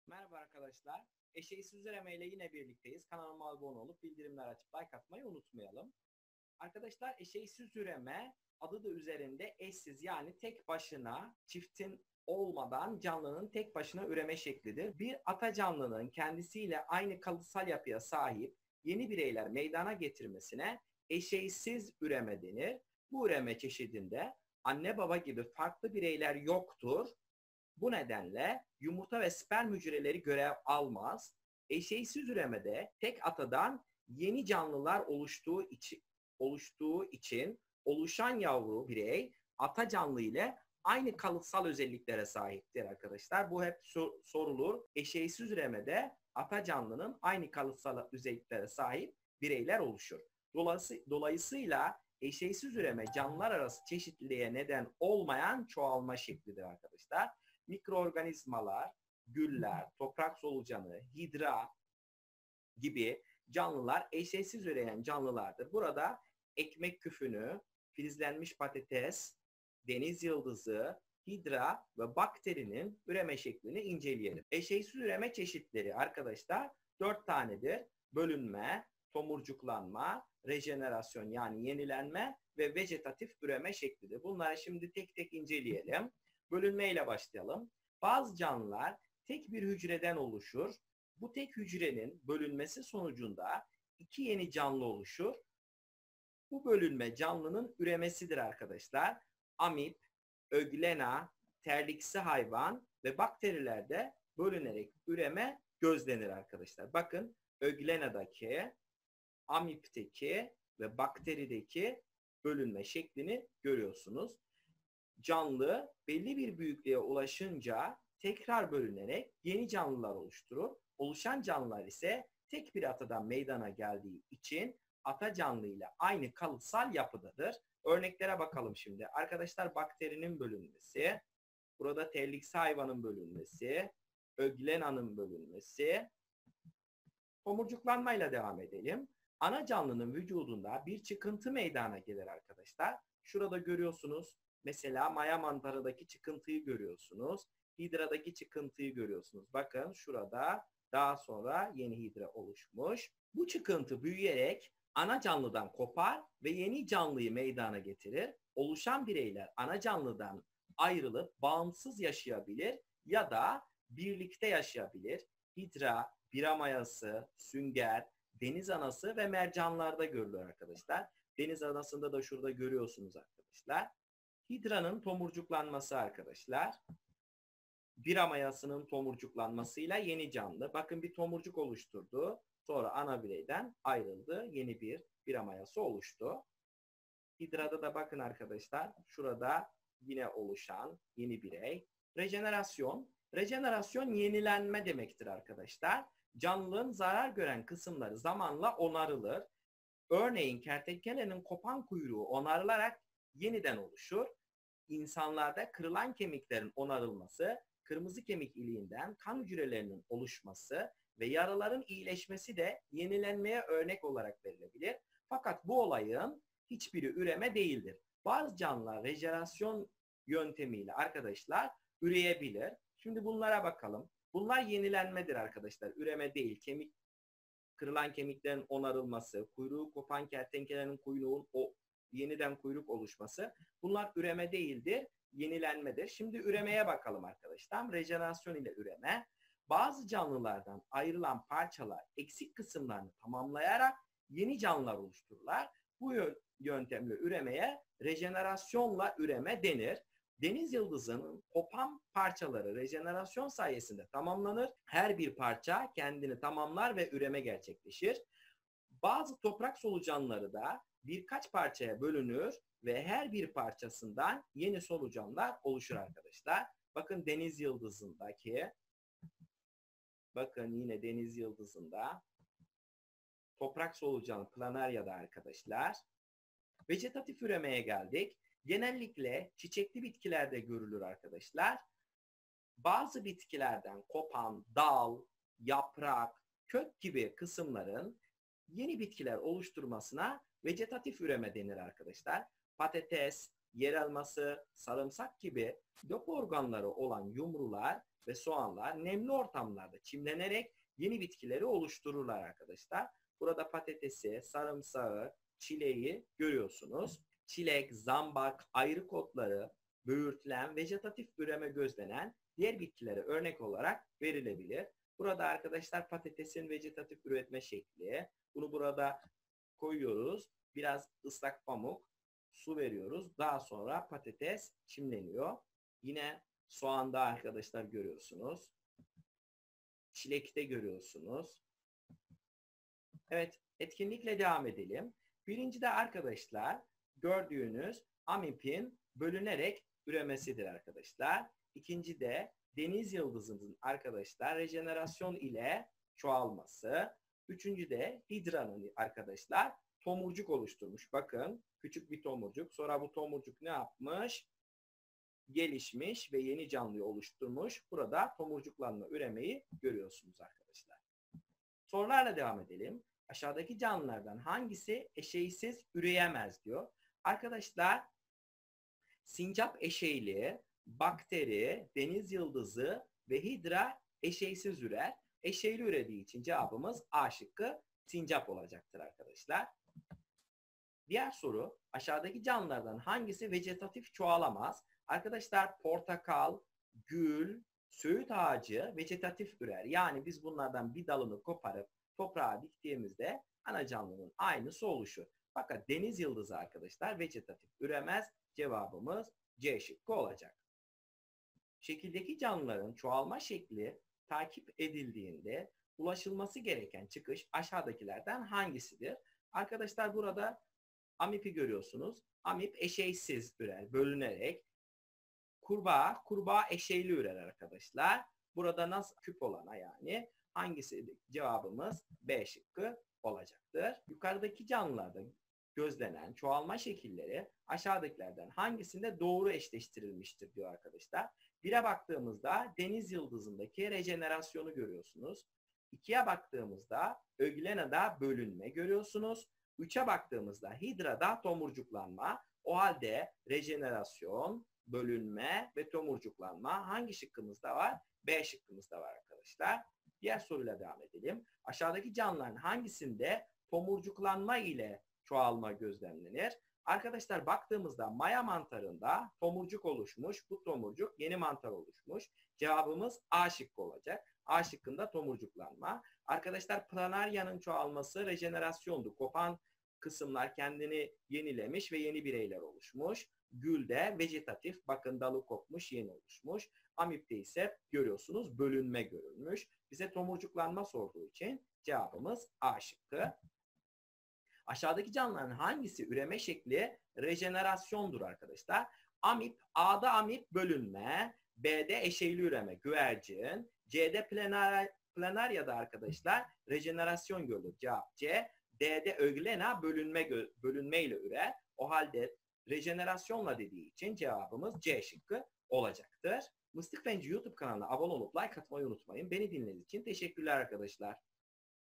7. SINIF EŞEYSİZ ÜREME
Merhabalar 7.sınıf eşeysiz üreme konu anlatımı ve soru çözümü. #7.sınıf